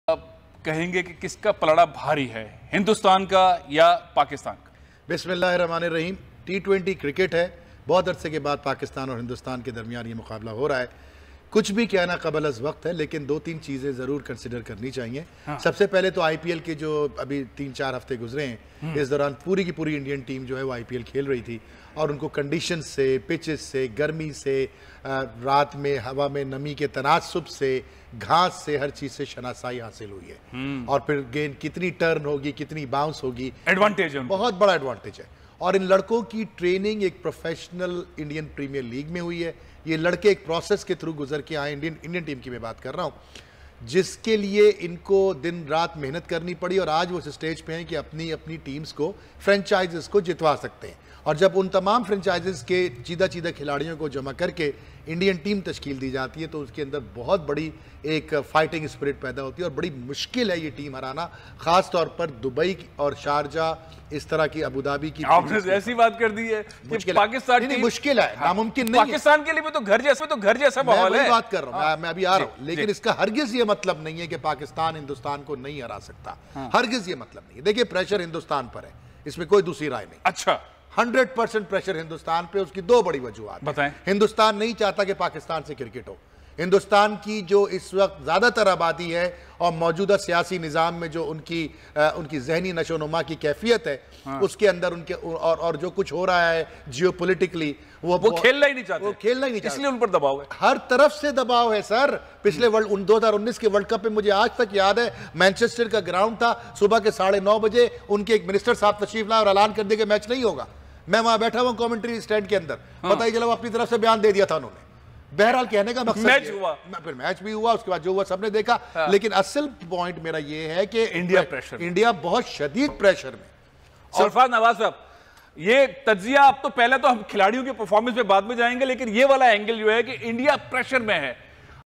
Pakistani news channel saying India is under pressure against Pakistan in World Cup Match
Pakistani news channel saying India is under pressure against Pakistan in World Cup Match. However, also accept India ka palda bhari hai